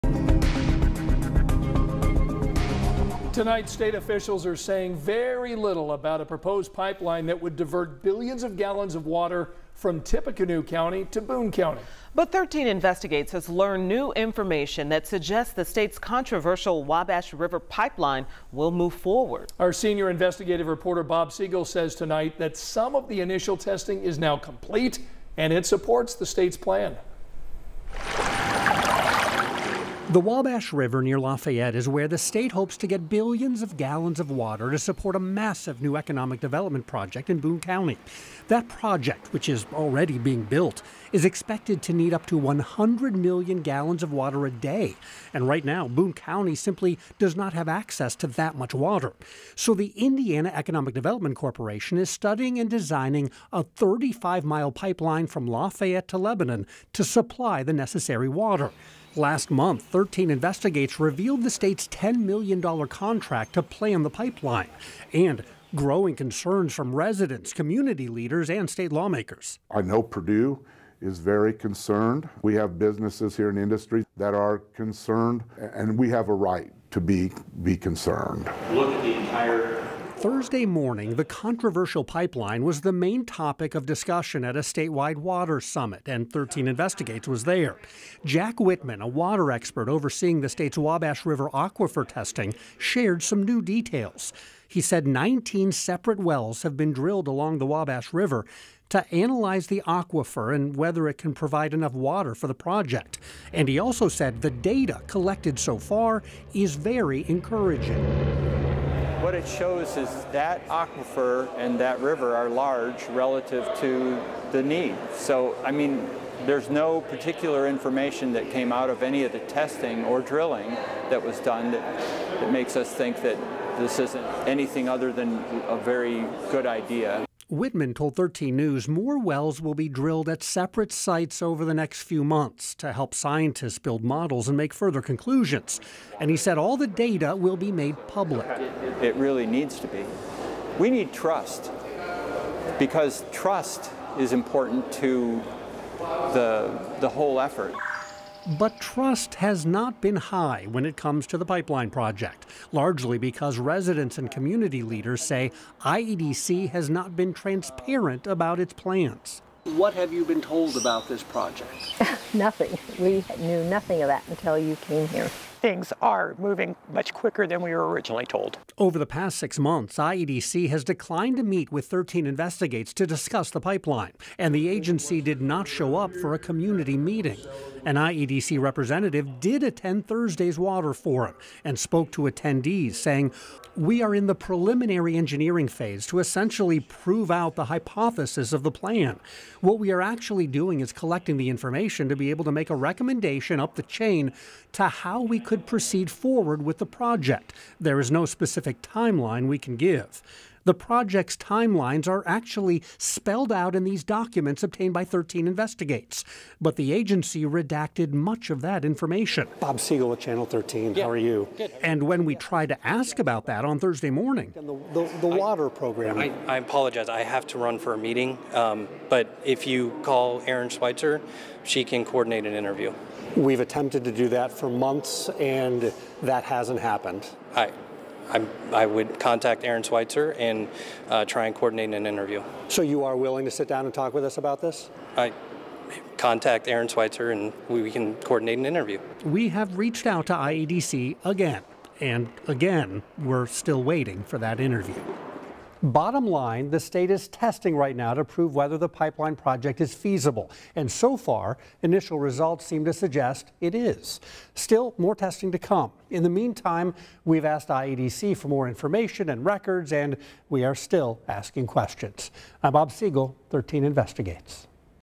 Initial tests for Lafayette-to-Lebanon water pipeline show 'exciting' results
Initial tests to determine whether the Wabash River can support an ambitious pipeline project that would supply water to Boone County have shown promising results.